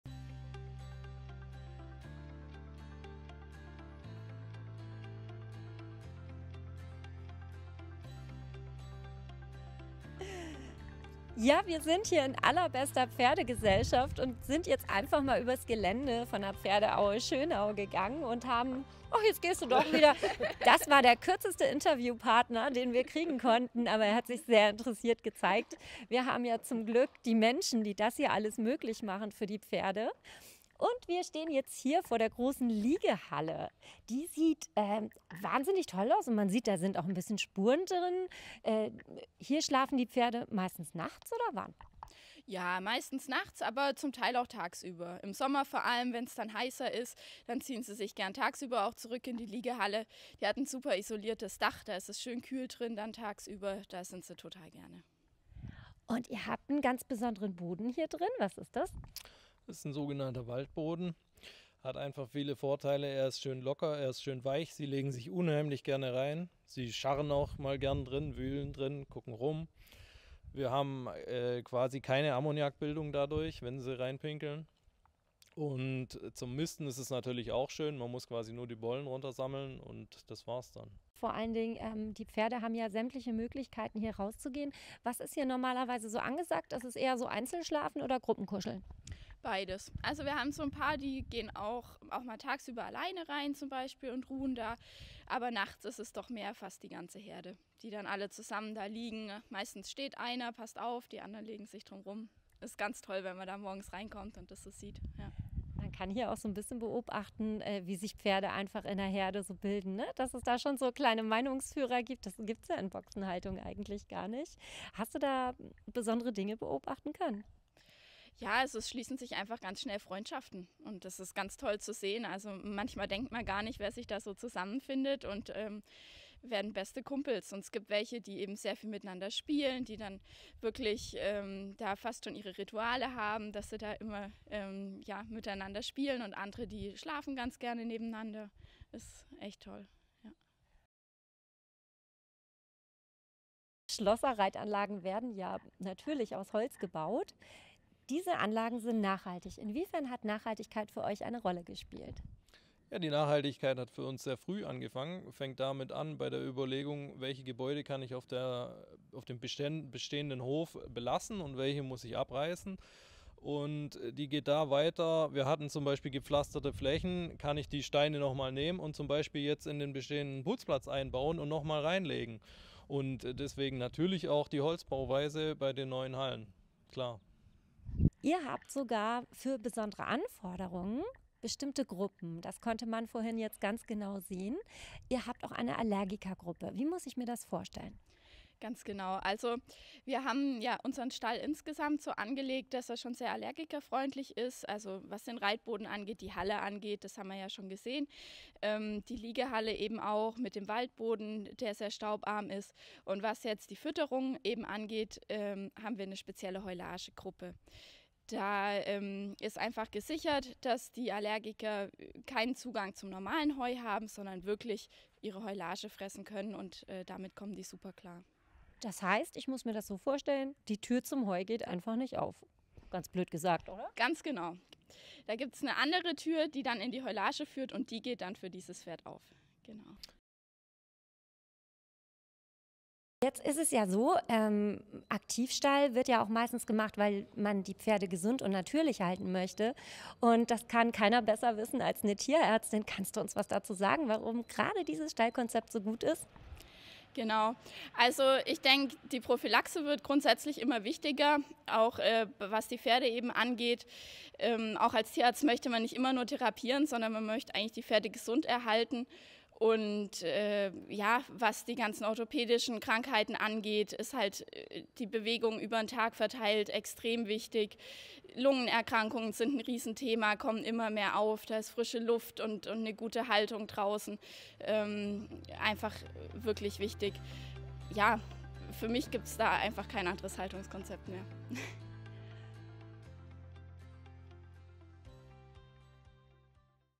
Pferdeaue Schönau: Aktivstall der neuen Generation
Anna & Chris von der Pferdeaue Schönau erzählen uns was das Aktivstall-Konzept ausmacht, welche Rolle Nachhaltigkeit beim Neubau Ihrer Reitanlage für sie gespielt hat und warum ein Aktivstall für sie die Haltungsform ihrer Wahl ist.